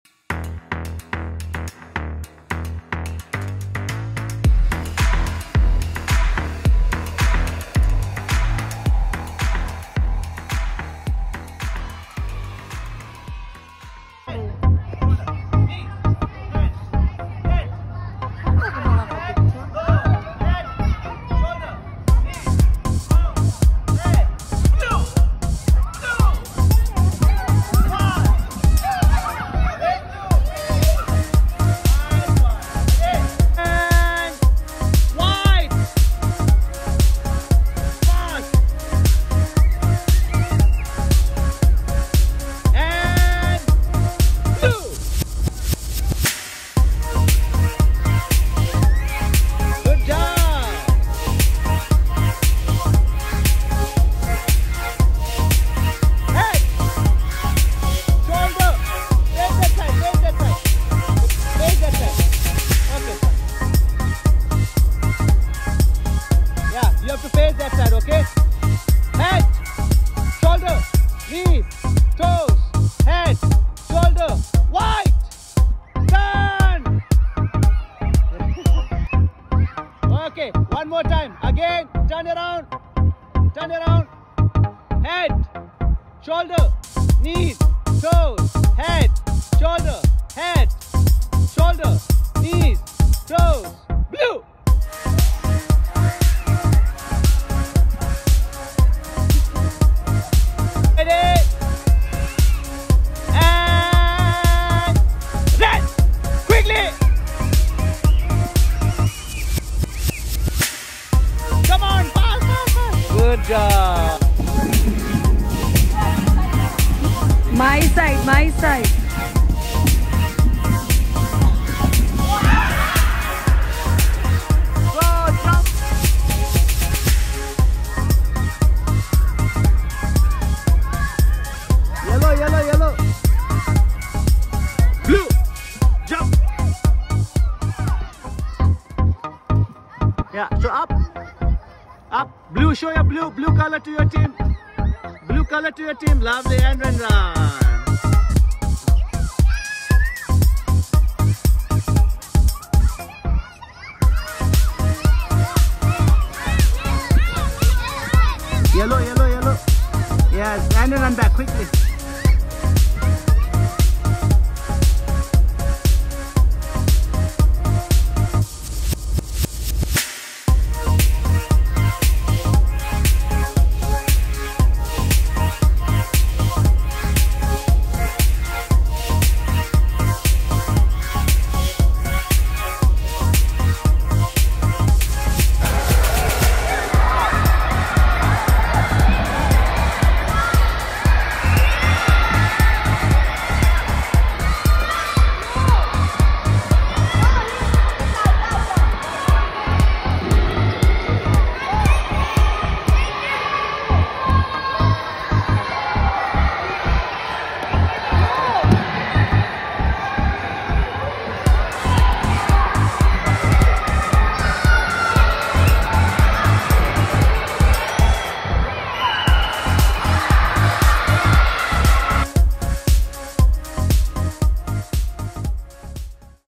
5 Fun Activities With a Colour Box || pegames || physed || physical education
1. Two groups of children face each other, each with a colored box placed in front of them. When the teacher calls out a body part, the kids must quickly touch that part. Similarly, when a color is called out, they must find the corresponding color as swiftly as possible before their peers. To add more challenge, all the children must turn around, showing their backs to the boxes. When a color is called, they must swiftly turn around to locate it on the box. Another variation involves children sitting, turning around, and promptly reacting to the color by standing up and turning around.

2. Kindergarten children participated in a color-finding activity using a box with different colors on each side. When a teacher called out a color, one child at a time would run or jump over obstacles toward the box to find the specified color. The activity promotes cognitive development by enhancing color recognition skills, encourages physical activity through running and jumping, and fosters listening skills as children respond to verbal cues from the teacher. Additionally, it promotes teamwork and social interaction as children take turns and collaborate in the activity.

3. The students will be split into multiple groups, with each group receiving 8 color boxes. Their objective is to arrange the colors on each side of the box similar to a Rubik’s cube. Once the command to start is given, all groups will begin arranging the colors. The first team to successfully solve it will be declared the winner.

4. Children participated in an engaging activity where two players compete to arrange six boxes horizontally. The goal is to ensure that the side facing their team displays the same color for all six boxes . This activity promotes the development of cognitive skills such as rapid decision-making, strategic planning, fine motor control, mindfulness , emotional regulation, and sustained attention among the participants.